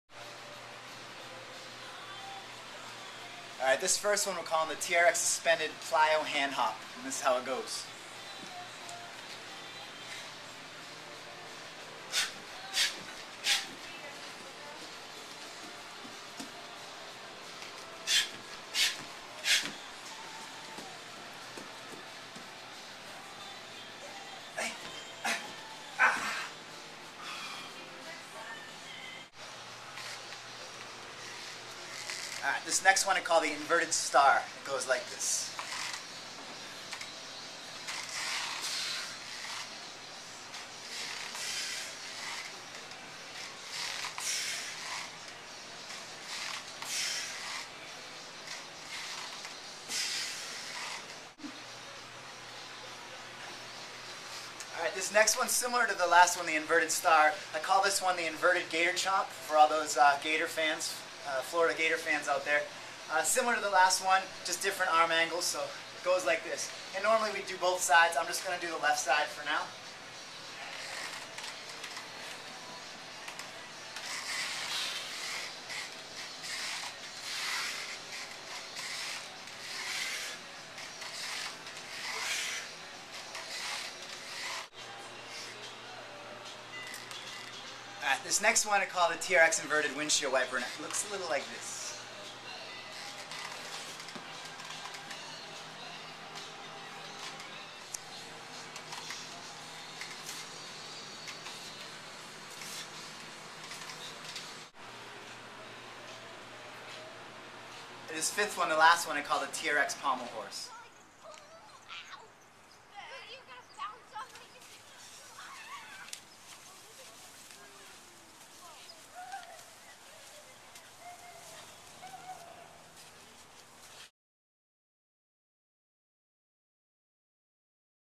5 TRX Moves You Need To See
Here are 5 TRX movements you may have never seen before. These are fairly advanced exercises that will definitely test your core strength and coordination. I do not recommend these for all clients so please be use caution when performing any of these movements.
1. TRX plyo hand hops
2. TRX inverted star
3. TRX inverted gator chomp
4. TRX inverted windshield wiper
5. TRX pommel horse